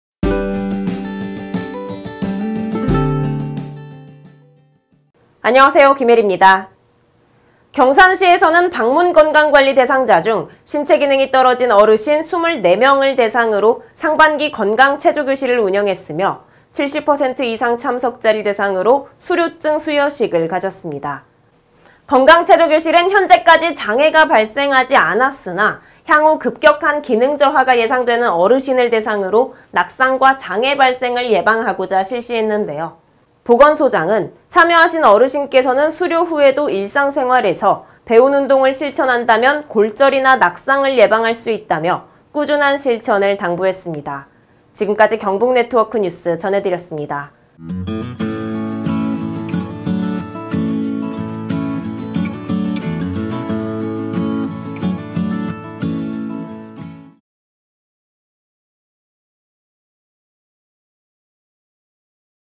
경산시청 어르신24명대상 상반기건강체조교실 최영조시장 경북경제발전위원회 경북포털 피폴tv 김애리기자
경산시에서는 방문건강관리 대상자 중 신체기능이 떨어진 어르신 24명을 대상으로 상반기 건강 체조교실을 운영했으며, 70%이상 참석자 24명을 대상으로 수료증 수여식을 가졌다.

  건강 체조교실은 현재까지 장애가 발생하지 않았으나 향후 급격한 기능저하가 예상되는 어르신을 대상으로 낙상과 장애발생을 예방하고자 실시했다.

  보건소장은 “참여하신 어르신께서는 수료 후에도  일상생활에서 배운 운동을 실천한다면 골절이나 낙상을 예방할 수 있다”며 꾸준한 실천을 당부했다.